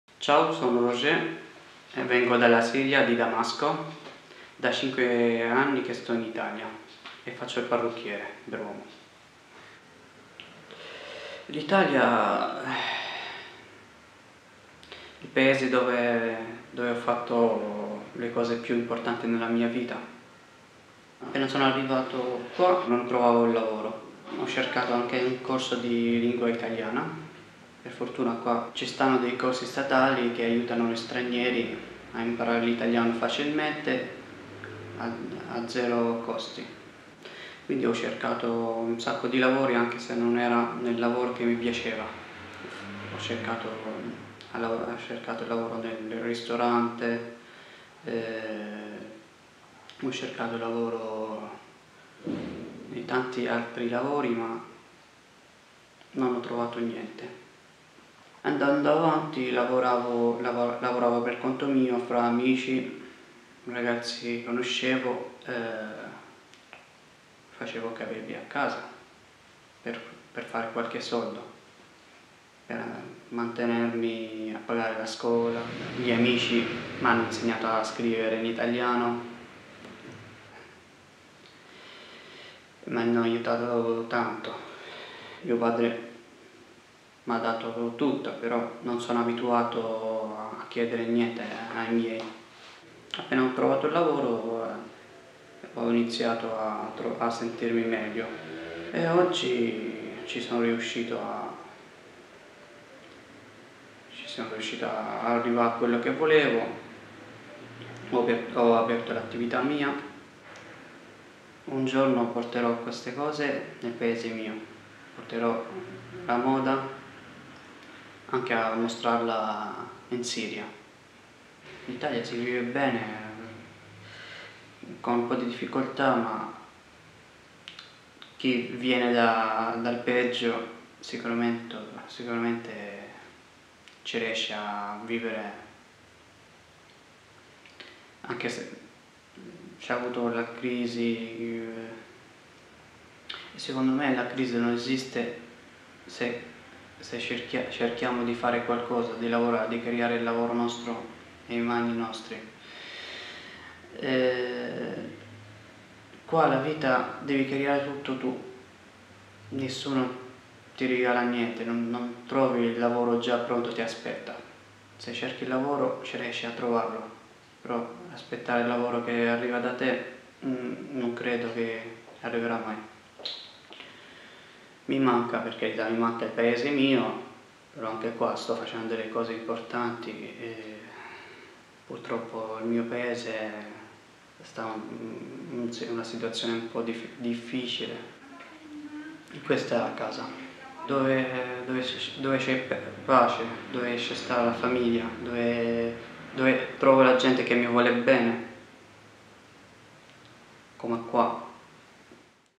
Rojeh e il lavoro ovvero Siria - Italia solo andata - Chronicles from the 20s
*Lavorare a vent'anni, oggi.* Continua il nostro viaggio in una delle questioni importanti per questa generazione: il lavoro. L'italia è il paese dove Rojeh Fares, siriano di 24 anni, ha fatto le cose più importanti nella sua vita. Uno sguardo molto diverso da quello precedente di Isabella, ma allo stesso tempo quasi il suo rovescio.
Chronicles from the 20s
Take a walk on the young side
Hopes, visions, future, fears of a generation.